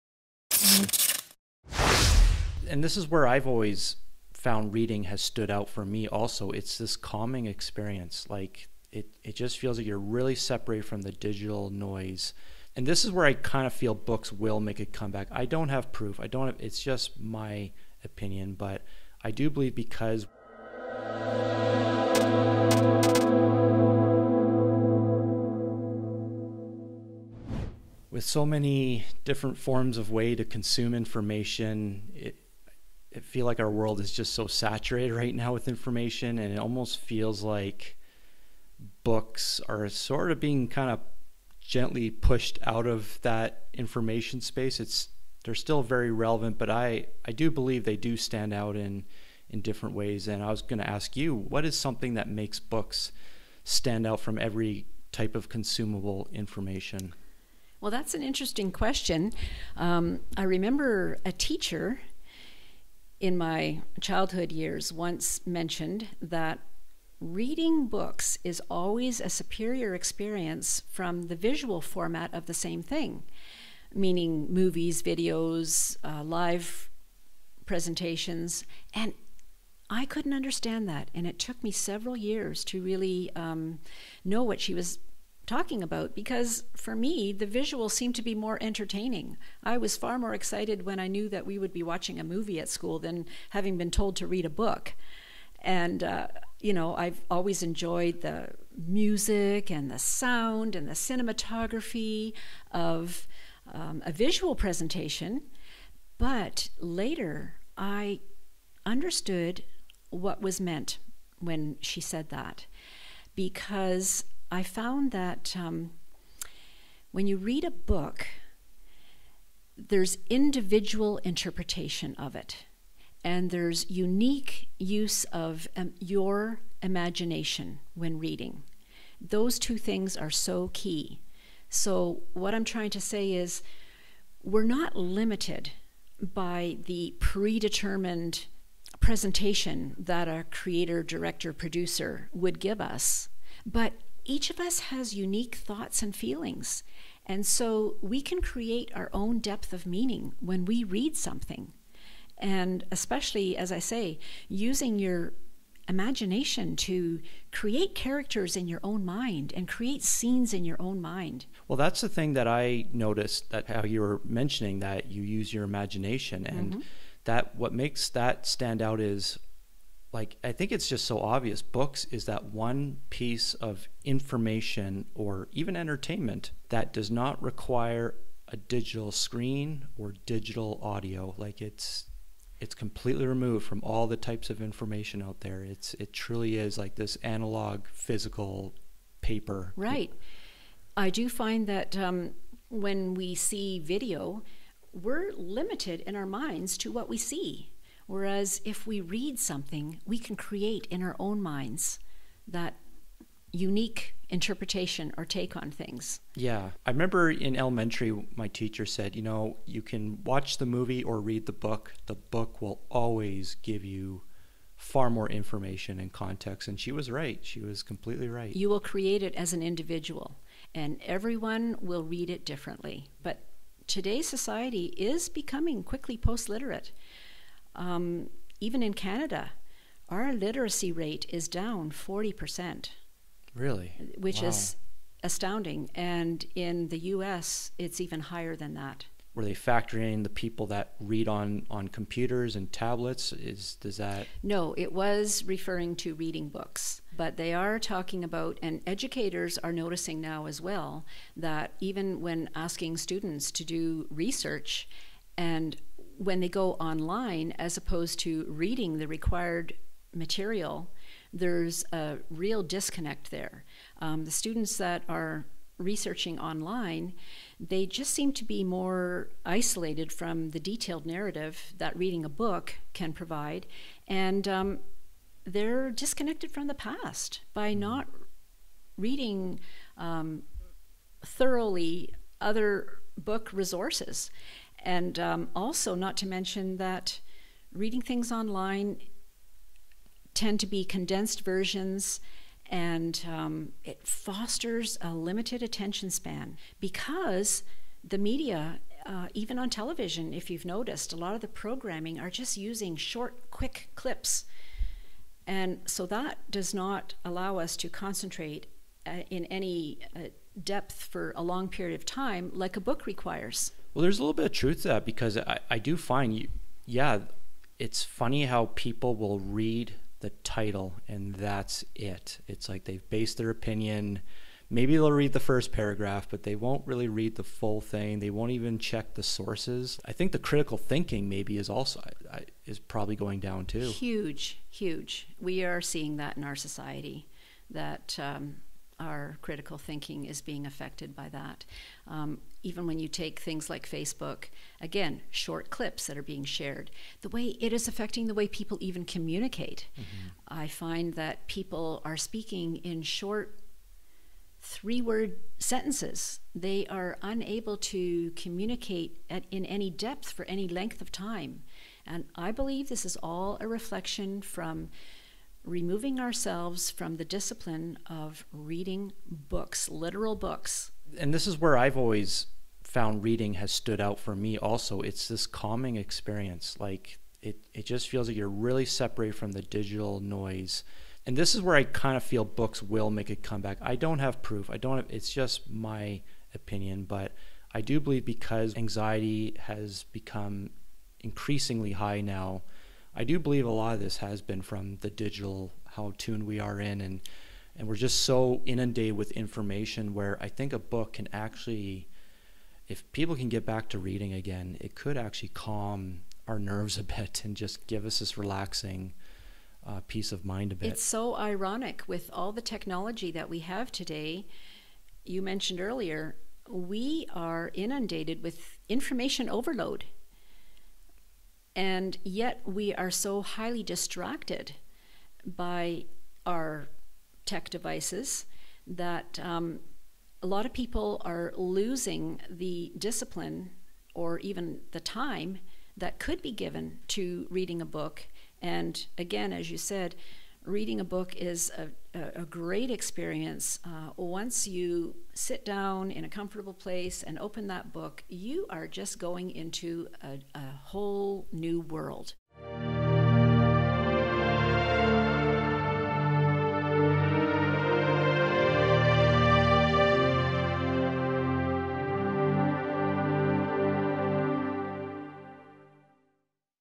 What Makes Books Stand Out? [ Conversations ]
Vera & Evan have a conversation about what makes books stand out from every other method of information consumption.